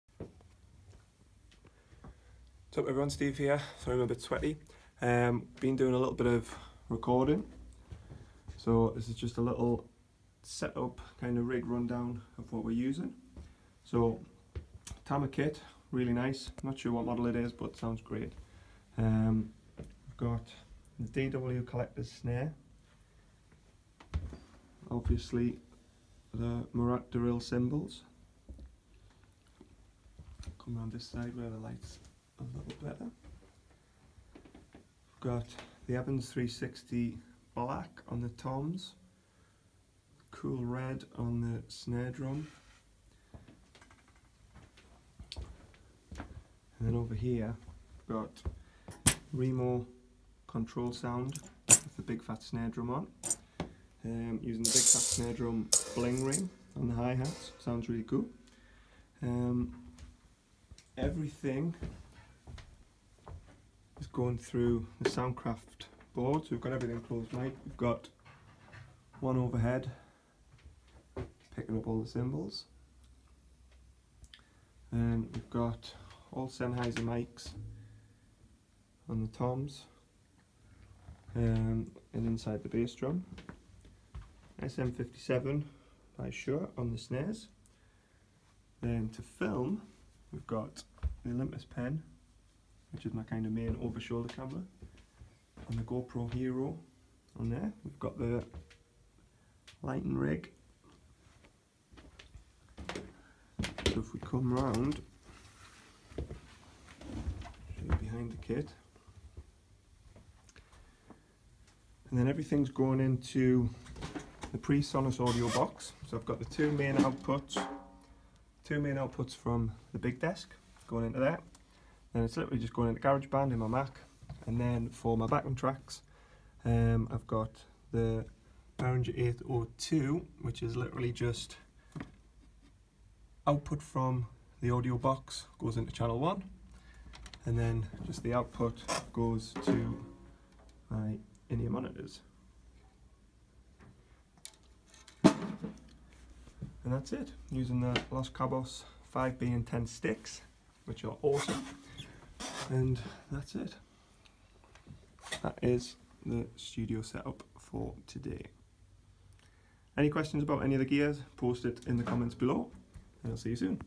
Drum Rig Rundown - Drum Kit and studio tour
Recording Studio Tour and drum kit review By Stephen Avison. See the drum gear I use and How to Record Youtube Videos.  Post Questions Below!

Recorded and Mixed by Stephen Avison at SoundInc Studios in Blaydon, UK.

IG, Twitter, FB @savisondrums